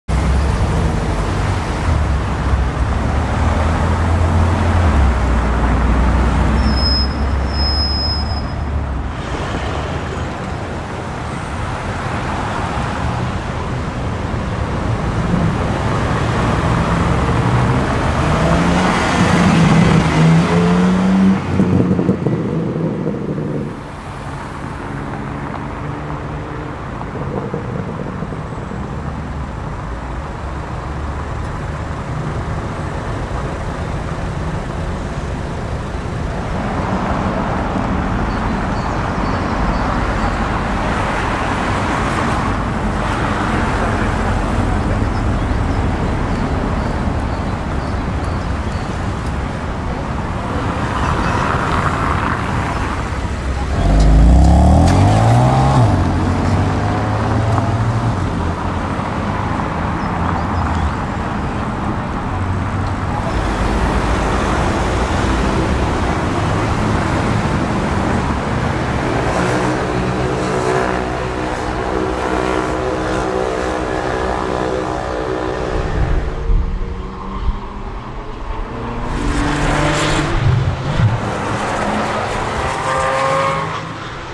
Different models of the BMW M3 | G82, F80, E46
Hello,
In this video you will see a 'BMW M3' in The Netherlands.
Cars in this video: BMW M3 G82, BMW M3 F80, BMW M3 E46 and a lot more!

I hope you will enjoy this video. Feel free to hit the 'Thumbs up' button if you like the video!